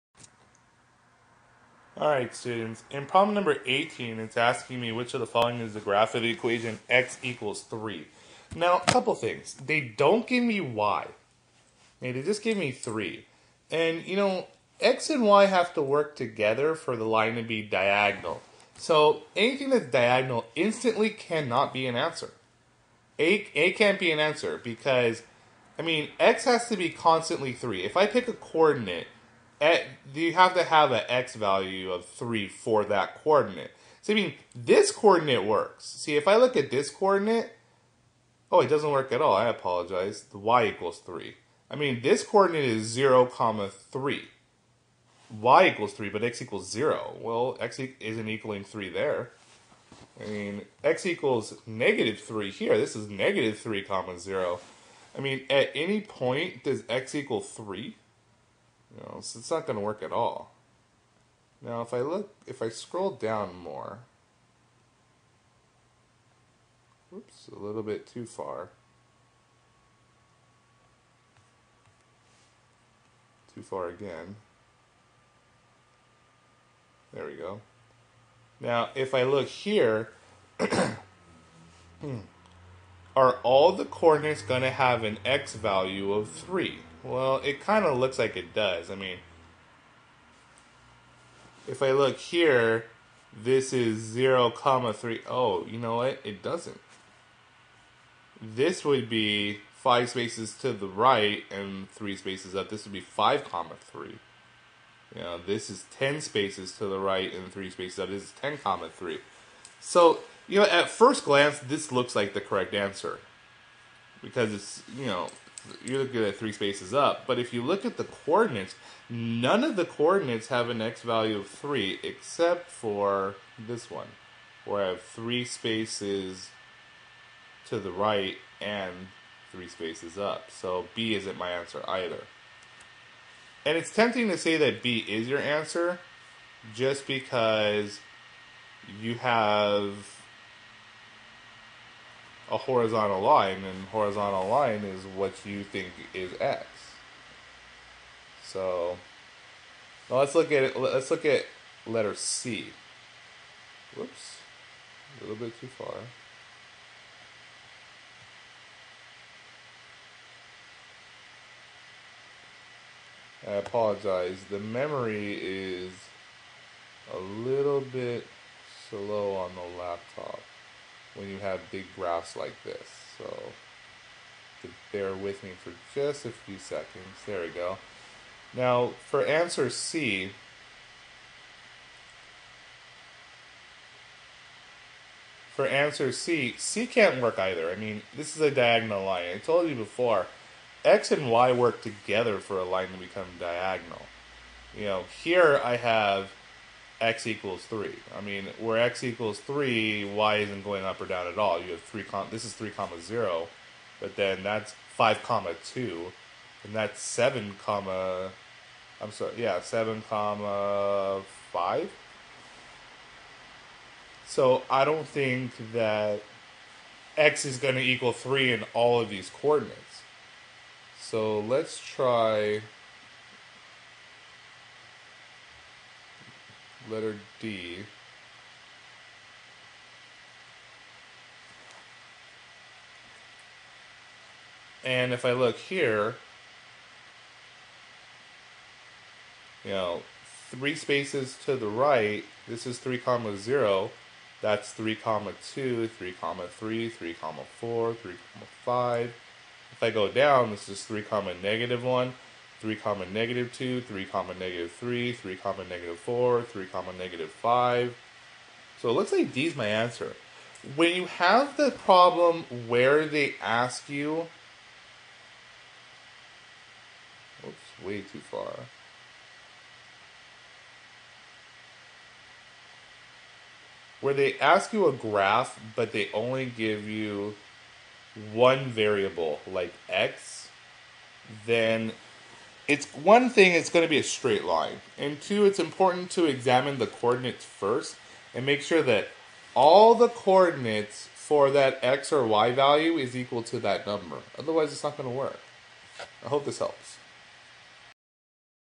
winter break assignment problem 18
mr danielczyk shows how to solve practice problem 18